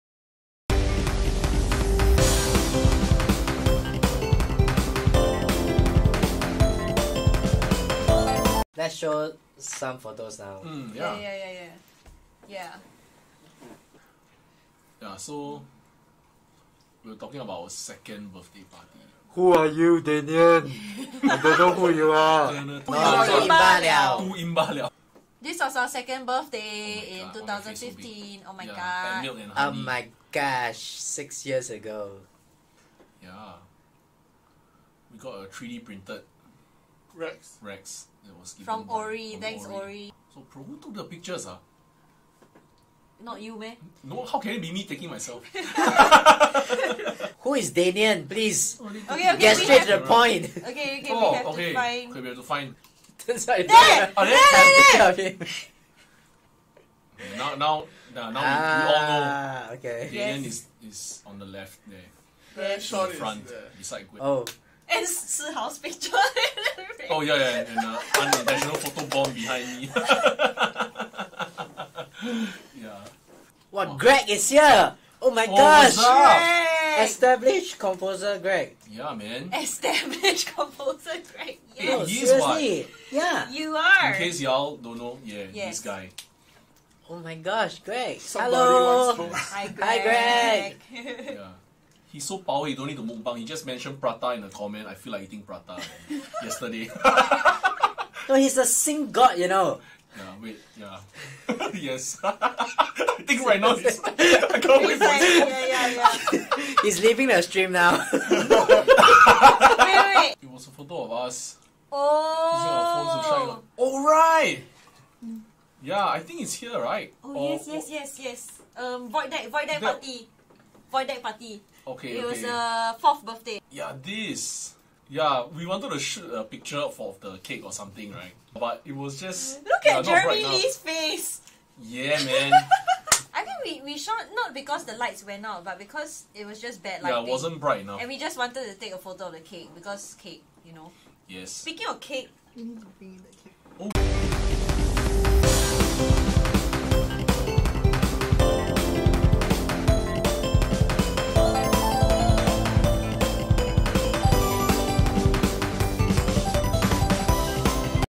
Imba Mukbang AMA Highlights - Past Birthday Parties!
Check out the highlights from 'The Interactive Imba Show (April 2021): Mukbang AMA 🤩

🥳 Imba turned 8 in April 2021!! Due to COVID situation, we had an internal celebration 🎂 + AMA + "Mukbang" on our Facebook Live! and here's the highlights from the livestream. Enjoy! To find out more abut what we REALLY DO, head over to our